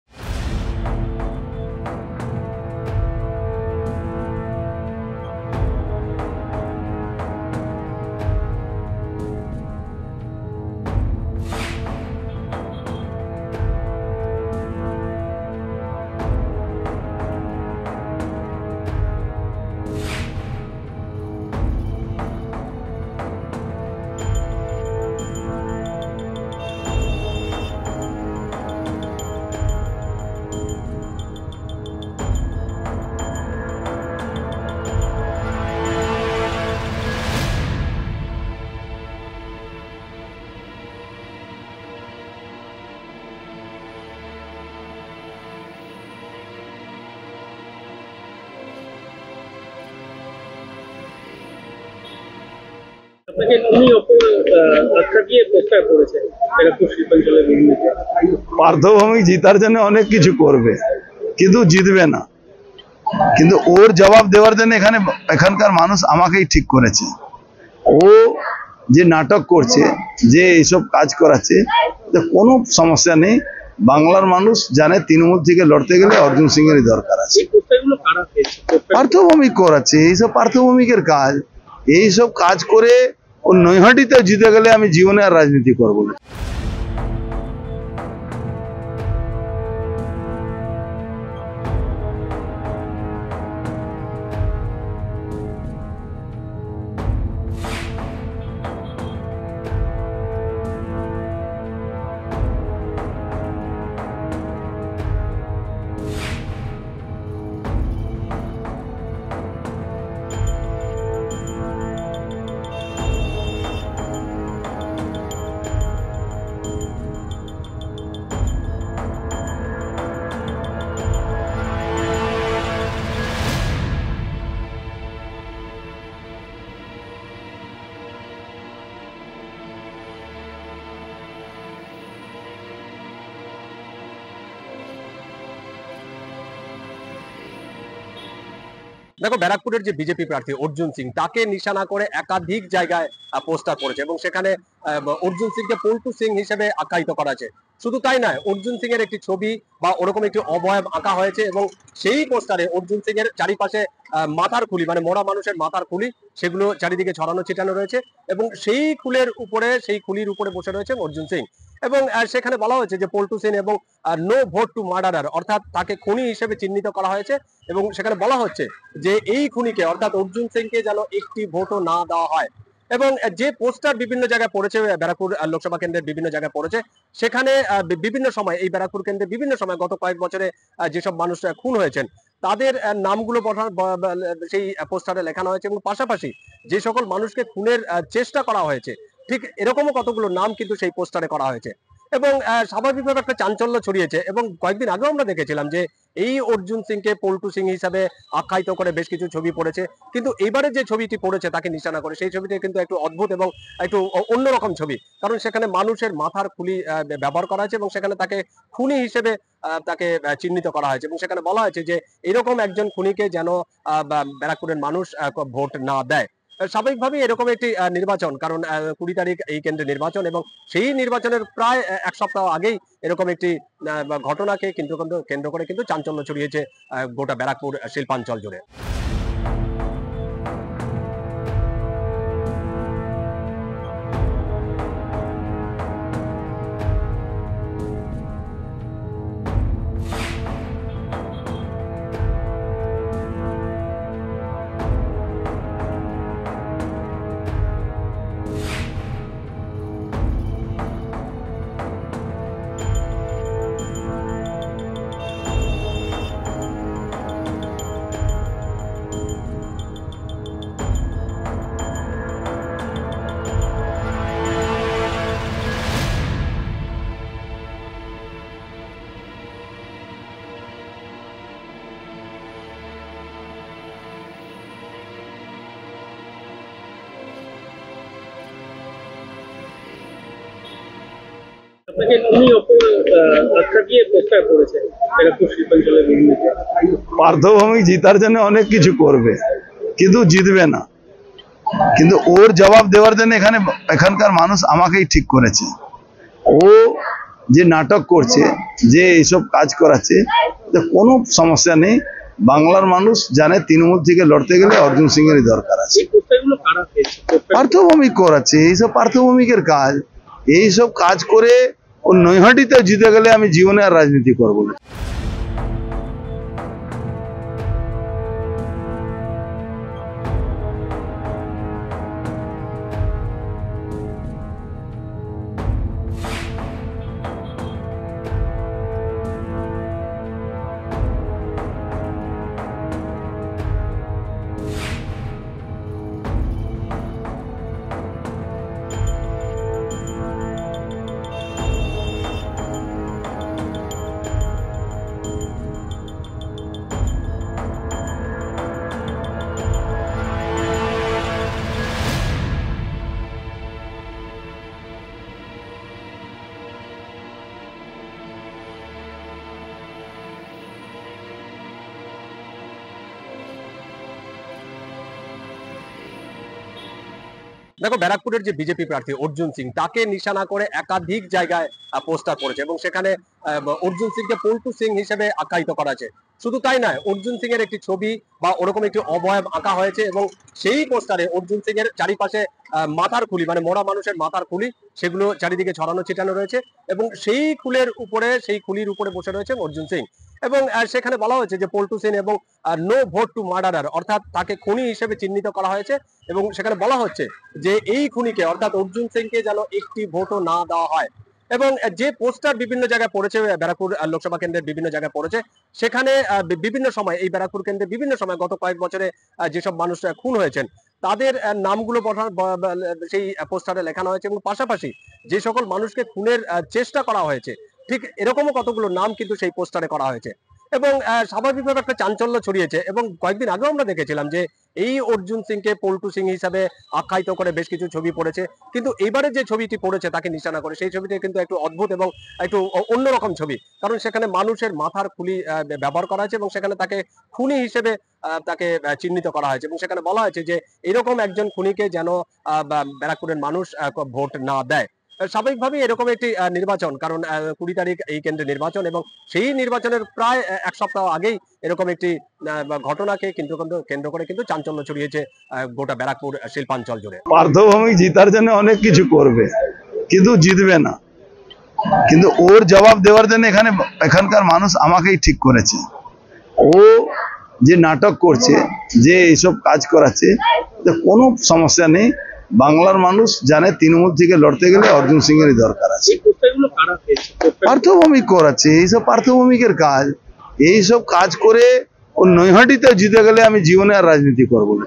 Breaking:অর্জুনের নামে 'খুনি পোস্টার' ব্য়ারাকপুরে! পার্থই মাস্টার মাইন্ড? Arjun Singh | Barrackpore
ব্যারাকপুর শিল্পাঞ্চলে অর্জুন সিং-এর নামে খুনি পোস্টার
অর্জুন সিংকে পল্টু সিং বলে ব্যাঙ্গ
পোস্টারে লেখা  'NO VOTE TO MURDERER'
অর্জুন সিং-এর দাবি পার্থ ভৌমিকই এইসব করাচ্ছে
'এইসব করে ও নৈহাটিতে জিতে গেলে আমি রাজনীতি করব না'